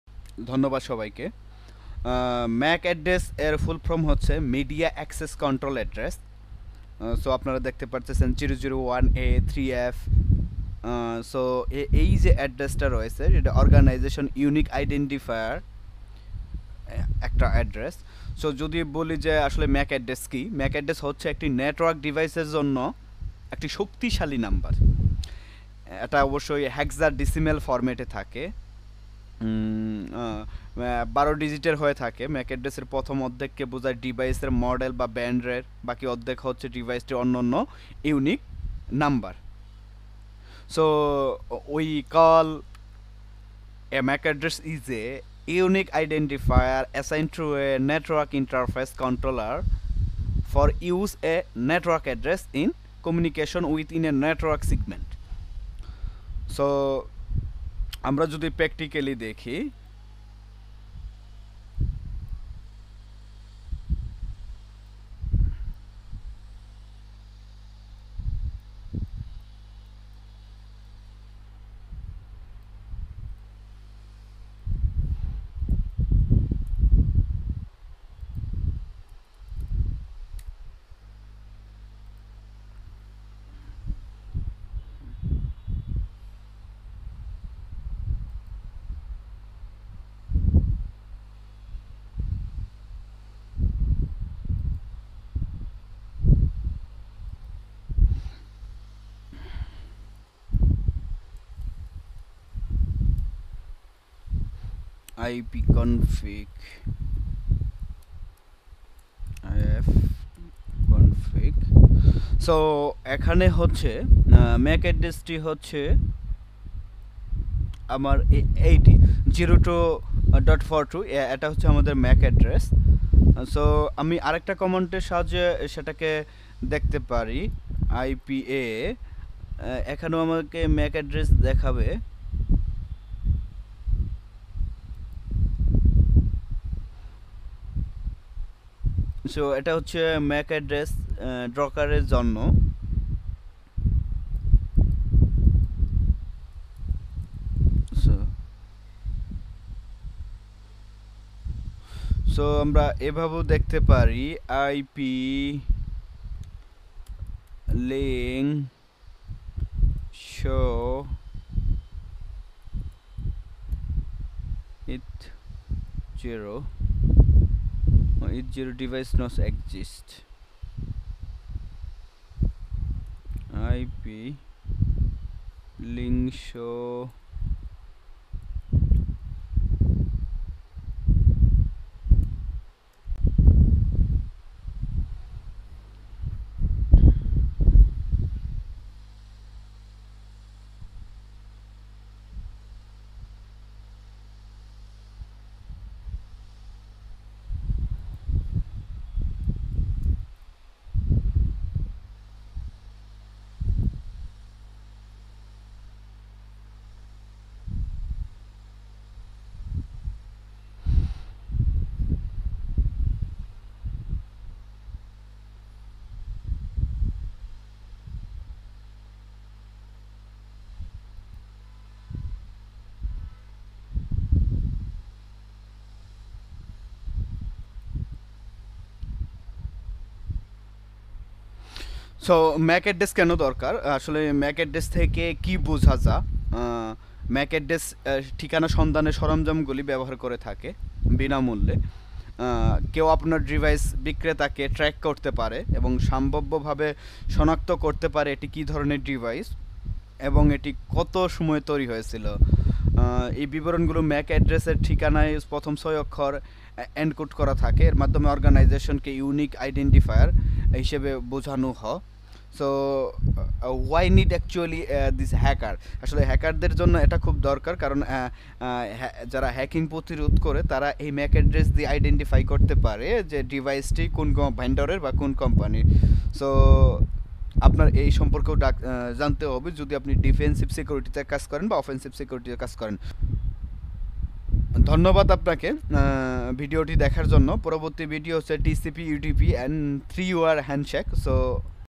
MAC Addresses || Cyber Security for Beginners || Boni Yeamin
Explore the world of MAC addresses in this informative video! MAC addresses play a crucial role in networking and are essential for device communication. In this video, we'll delve into what MAC addresses are, how they work, and why they're important. Whether you're a tech enthusiast or a networking newbie, this video will provide valuable insights into MAC addresses and their significance in the digital realm. Don't miss out on this essential knowledge!